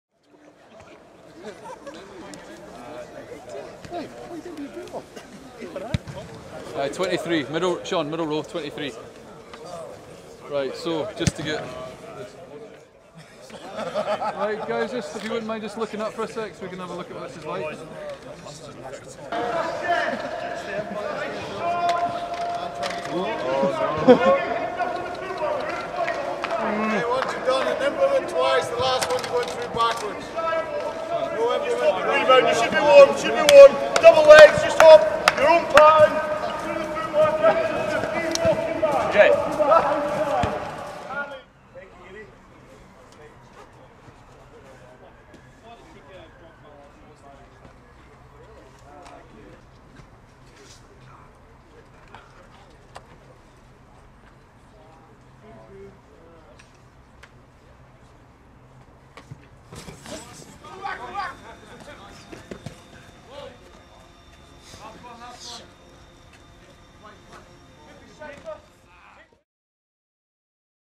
Scotland Training Camp | 3 days until Australia
We are back at the Oriam for another training session, plus we head over to Edinburgh University to catch up with the Australia squad ahead of the match this Saturday